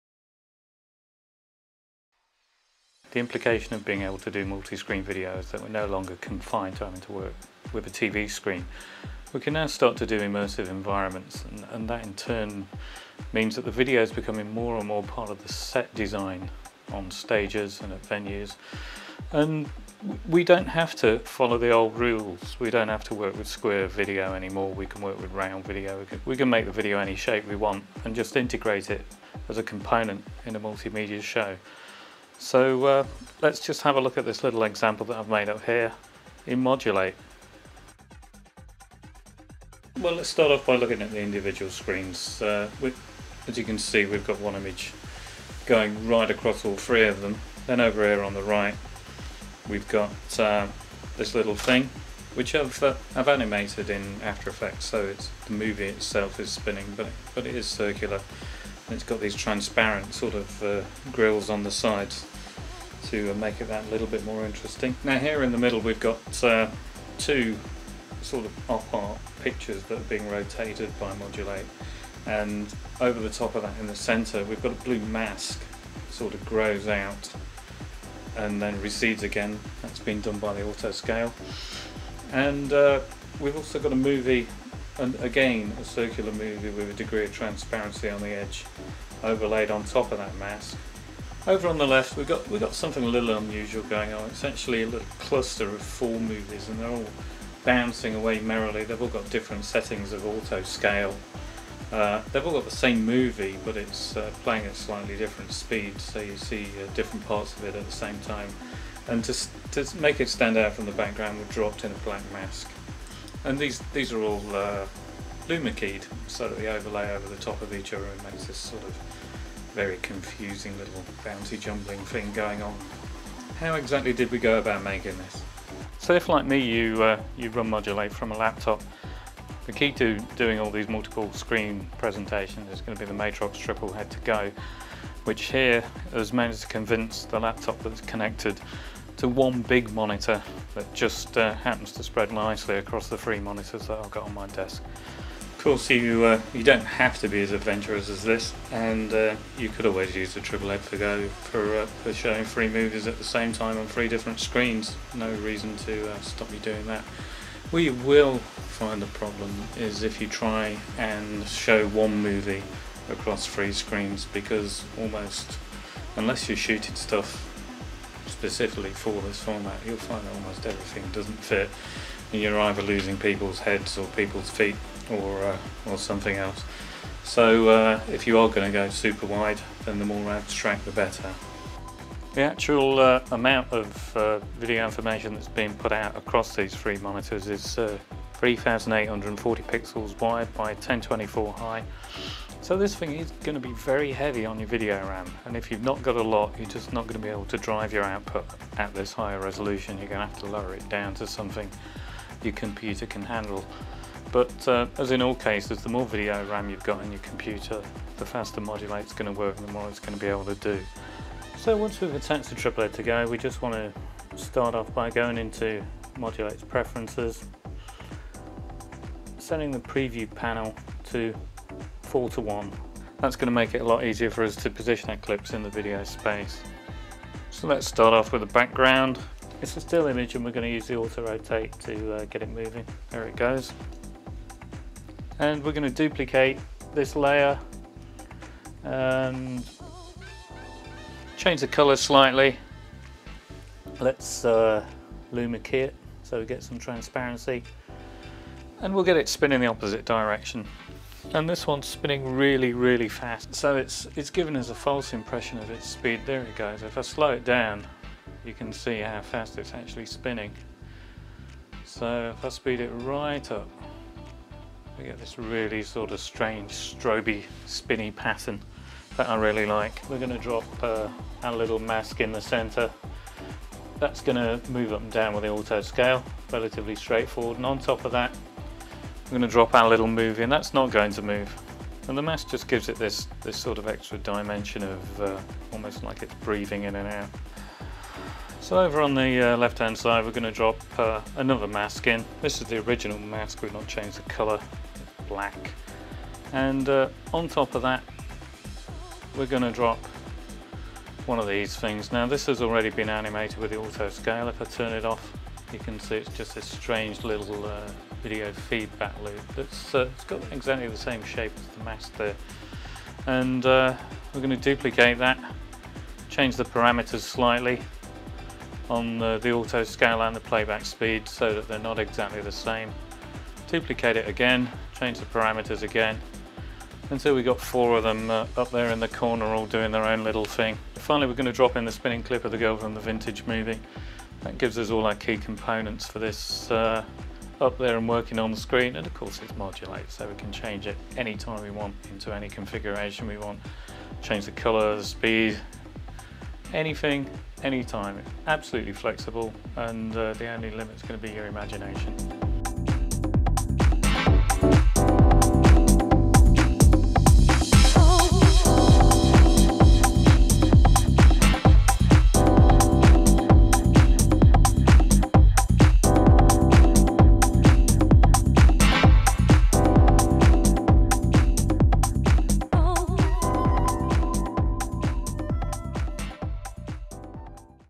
Modul8 multiscreen output
Using the Matrox Triplehead2go with Modul8